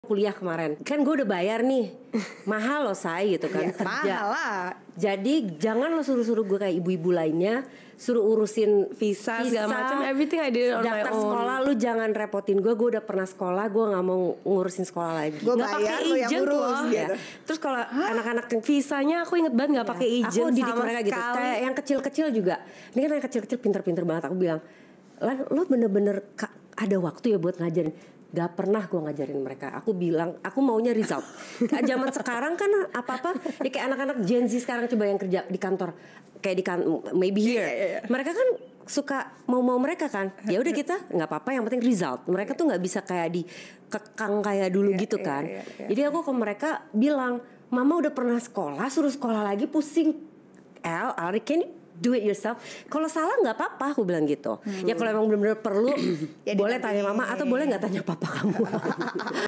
ANAK-ANAK WULAN GURITNO URUS KEPENTINGAN SEKOLAH SENDIRI⁉️
Saksikan keseruan episode TS Talks spesial Hari Ibu bersama Wulan Guritno & Shalom Razade hanya di TS Media Youtube Channel!